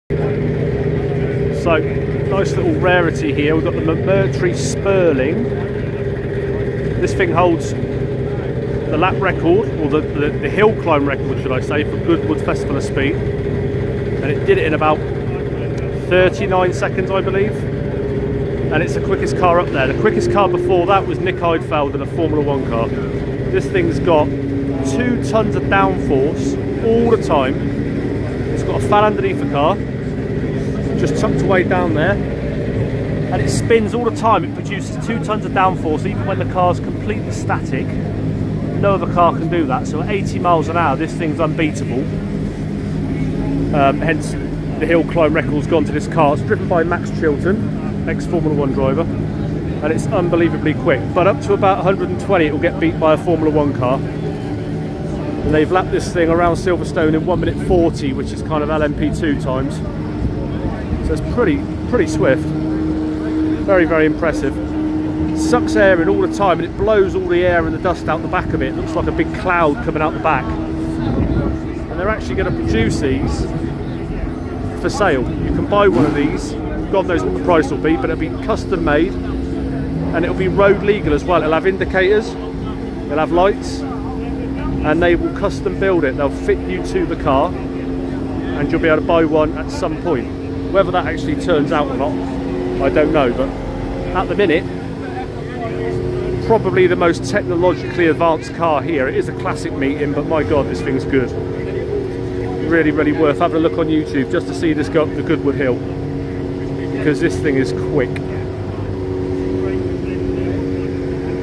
The McMurtry Automotive Spierling at the Silverstone Classic 2022 and FOS with Max Chilton
This is the famous " fan car " , 2 Tons of downforce even at a standstill and unbeatable in acceleration, a real technological masterpiece.